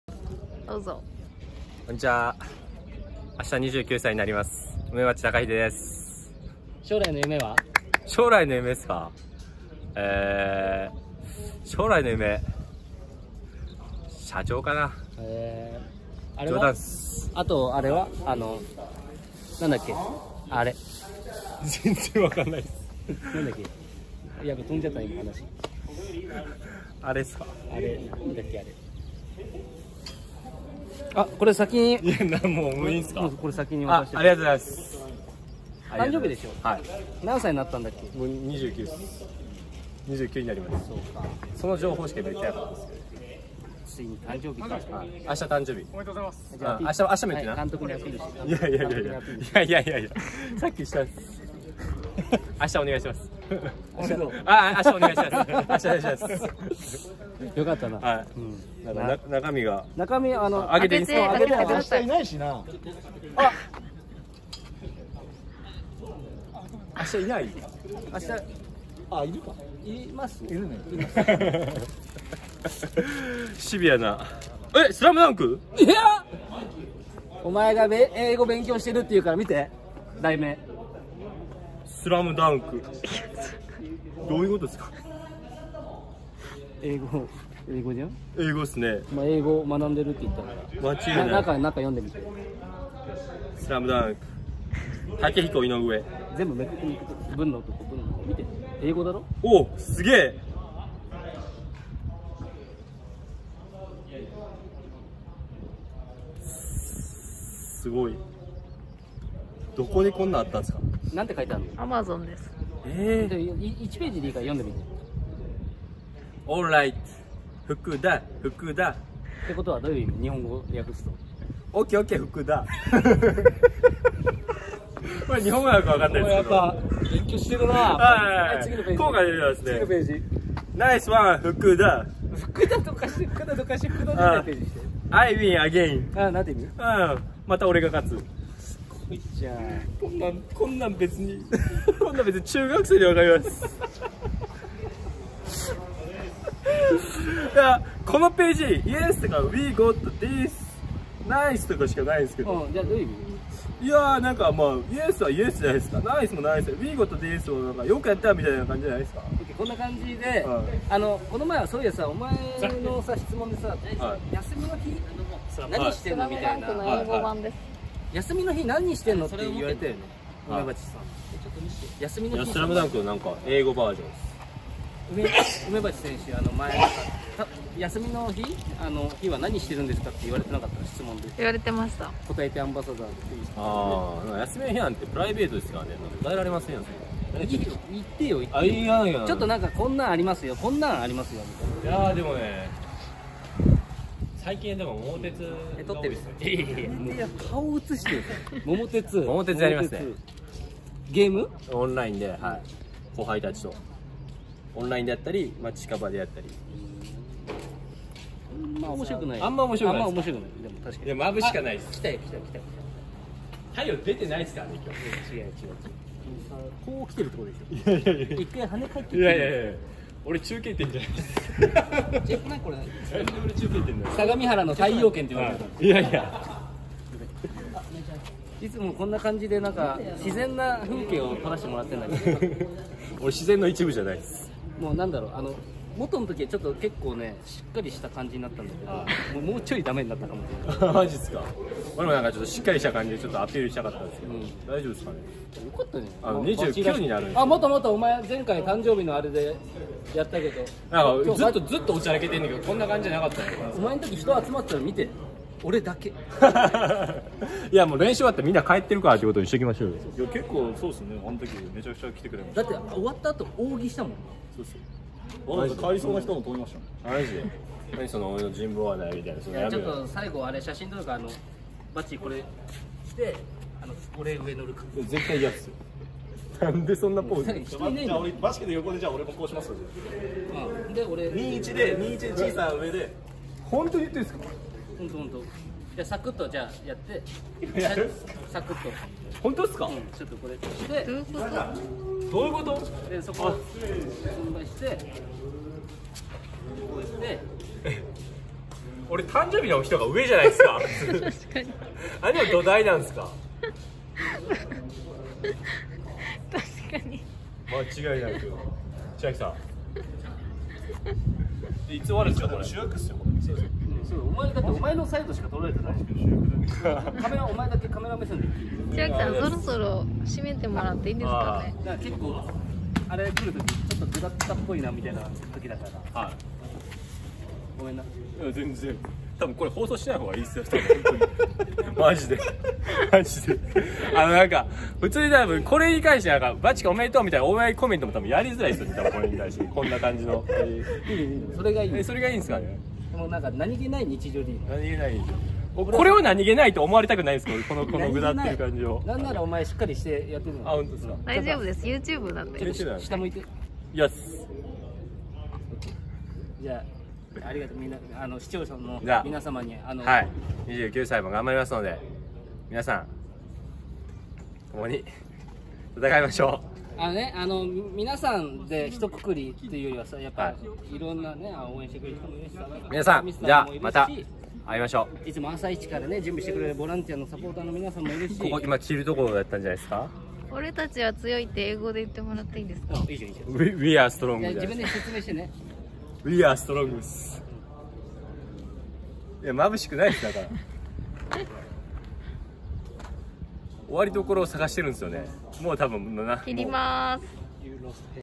【SC相模原】千明アンバサダーから誕プレ（MF33梅鉢貴秀編）
６月８日に２９歳の誕生日を迎えた梅鉢選手！
その前日にアンバサダーの千明さんから誕生日プレゼントをお渡ししました！

英語を勉強中の梅鉢選手に
梅鉢選手の大好きなスラムダンクの英語版漫画をプレゼント！

※非常にゆるい内容となっております。お時間のある時にぜひご覧くださいませ